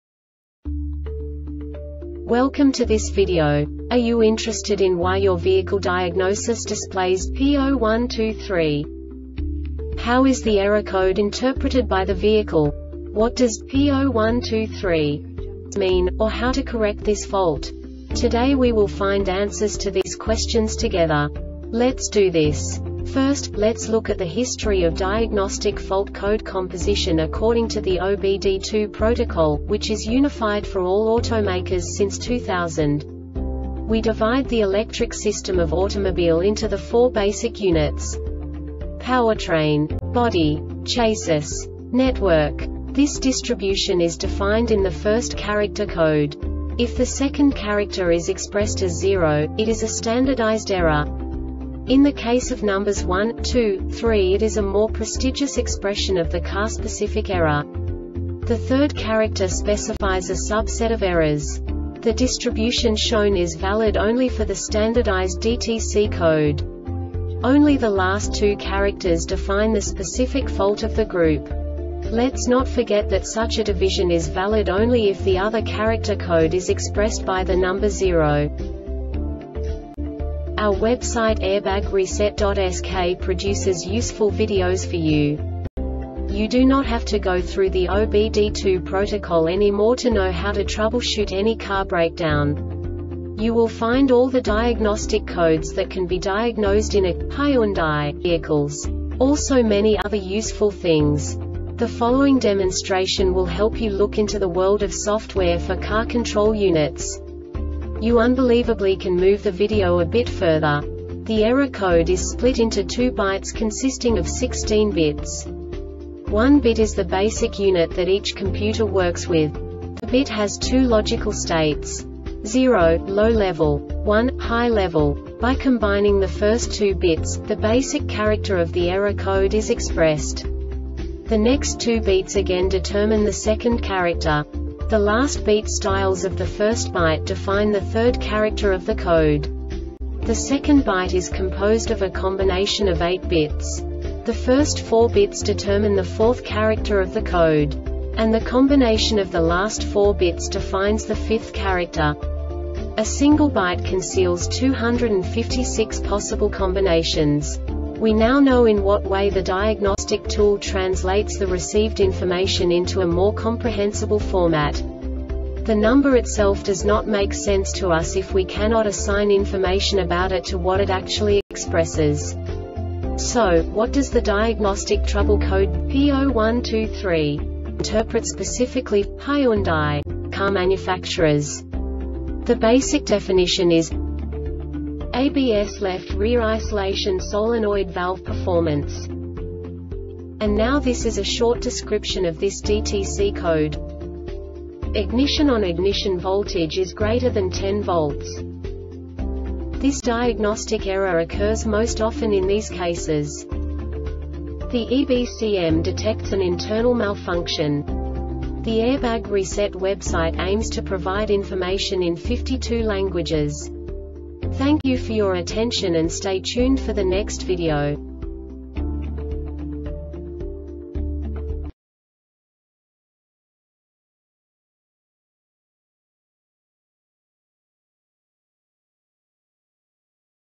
DTC Hyundai P0123 Short Explanation
Contents:
0:21 Basic DTC analysis according to OBD2 protocol standard.
1:48 Insight into programming
2:58 A diagnostically understandable description of the Diagnostic Trouble Code
3:05 Basic error definition

Make:
Hyundai
Code:
P0123
Definition:
ABS Left Rear Isolation Solenoid Valve Performance
Description:
Ignition ON.Ignition voltage is greater than 10 V.